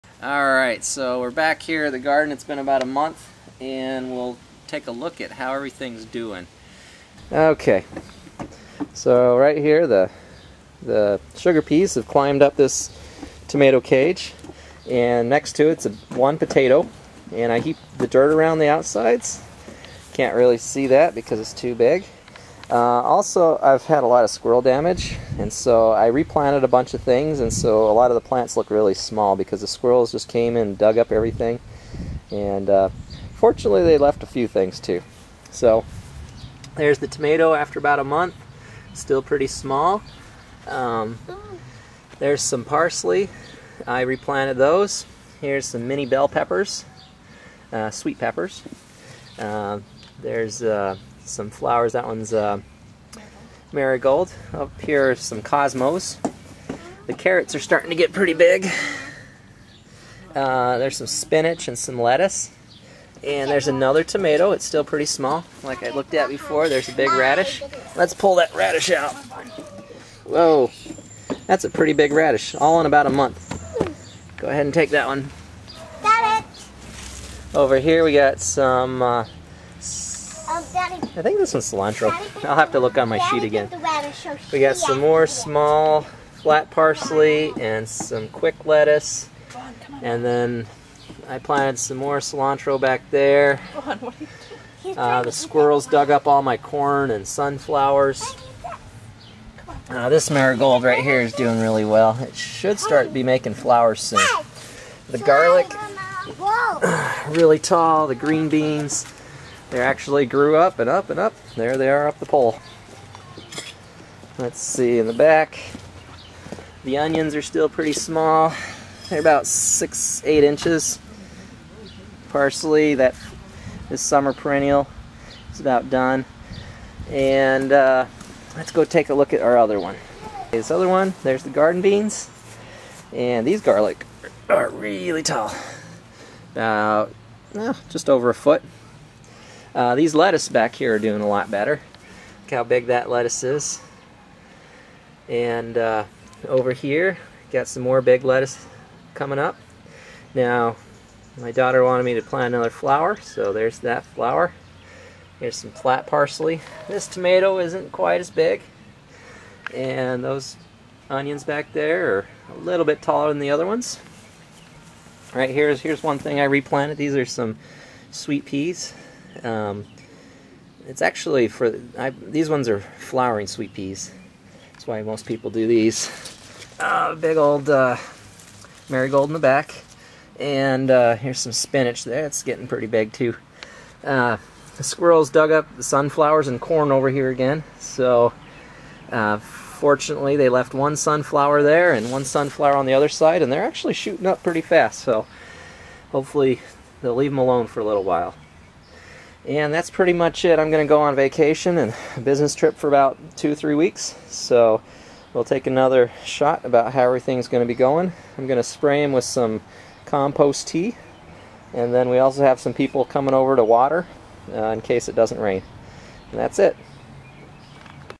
My square foot garden update June 17, 2013
Just an update on my garden. I will be gone for the next three weeks on a business trip and vacation and wanted to chart how things are growing. I will spray everything with compost tea and I have some one to come by to water if it doesn't rain.